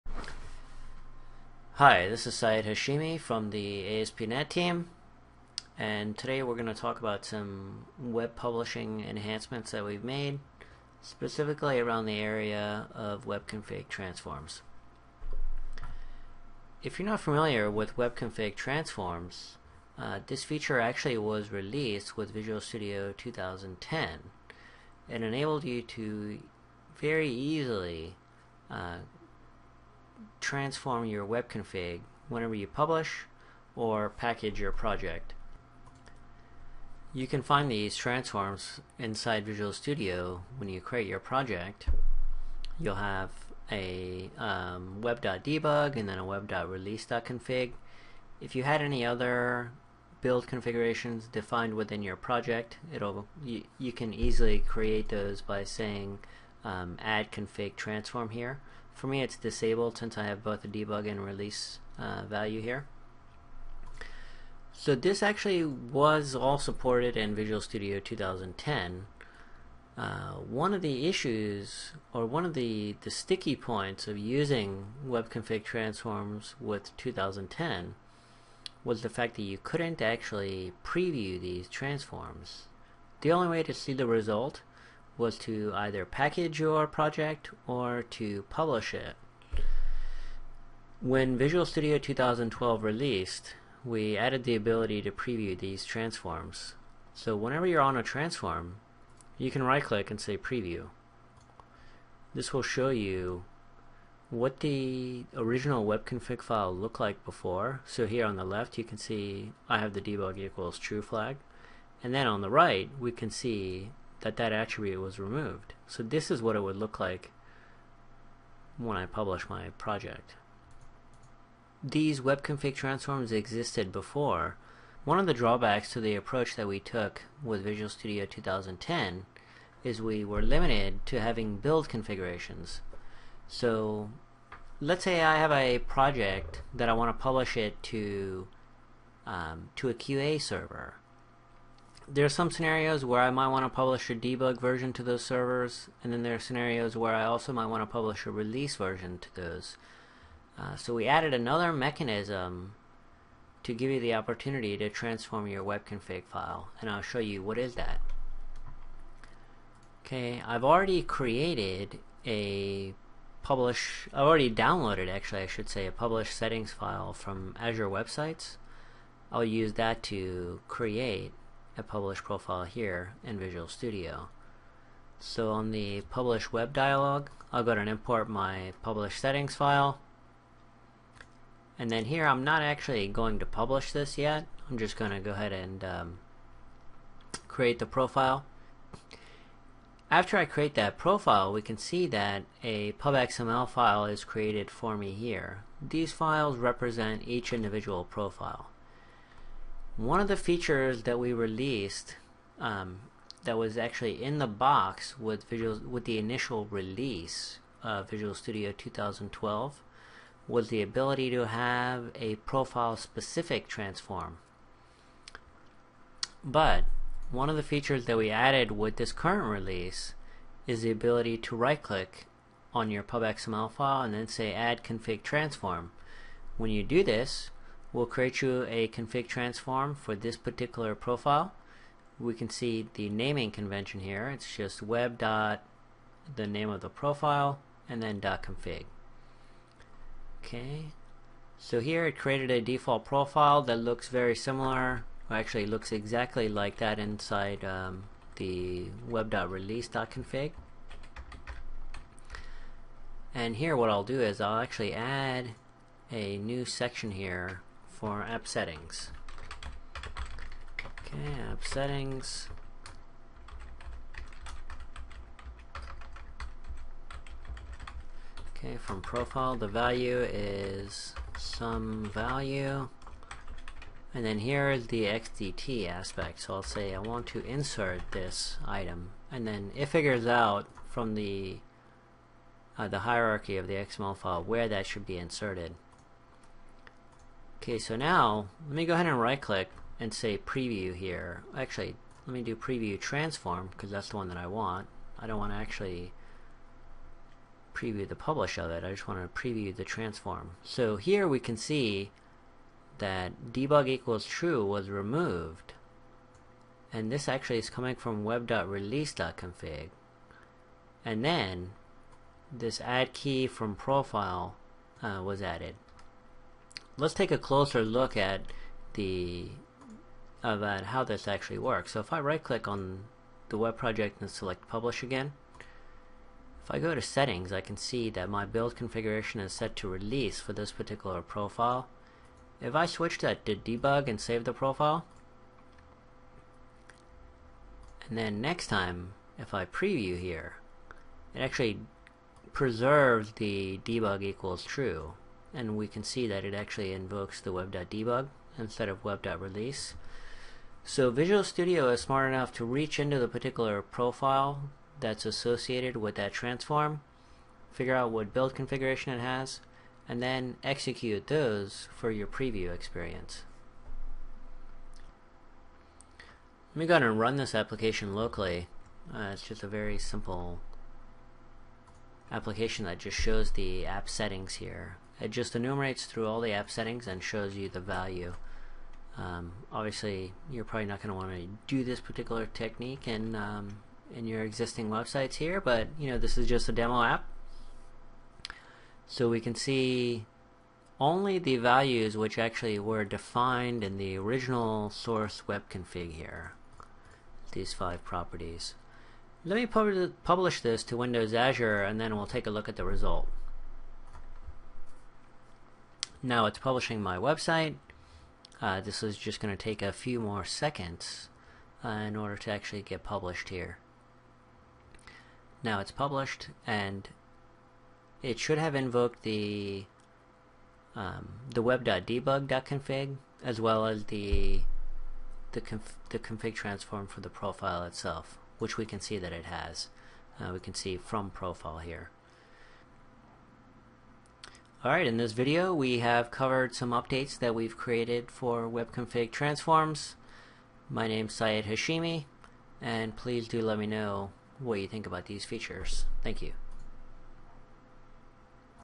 Web Tools 2012.2 - web.config transforms
This video shows some recent updates to the Visual Studio web publish process. This video covers the profile specific web.config transforms as well as other related features.